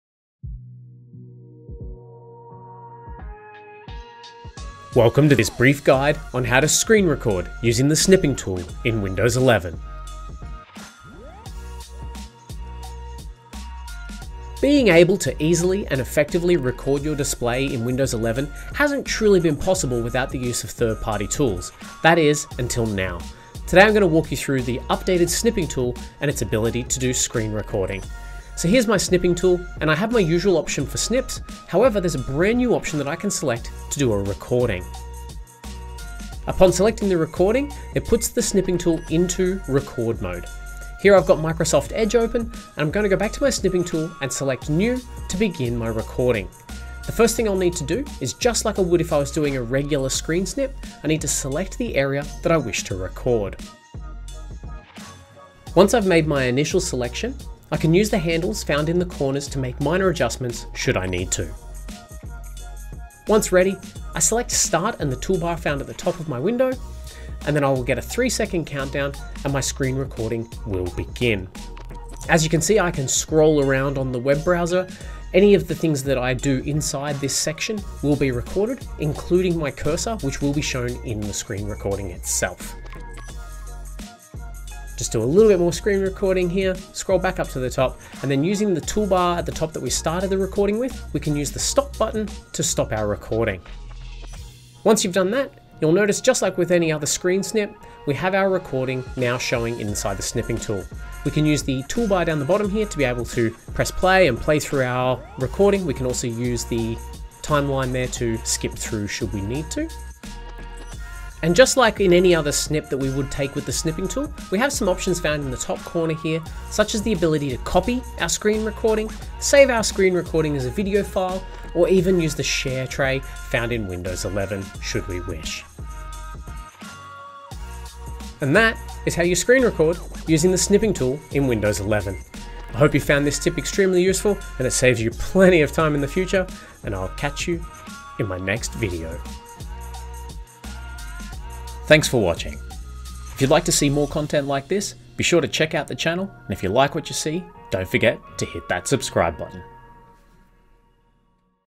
Record Your Screen Effortlessly with Windows 11 Snipping Tool
Capture every moment of your screen with ease using Windows 11 Snipping Tool. Get started with a step-by-step guide on how to record your screen in just a few clicks.

- What is the Snipping Tool: Discover the Snipping Tool, a powerful feature in Windows 11 that allows you to take screenshots and record your screen.
- Recording Your Screen: Learn how to use the Snipping Tool to record your screen in Windows 11, including how to set up and adjust recording options.
- Sharing and Saving Your Recordings: Find out how to share and save your recordings with the Snipping Tool.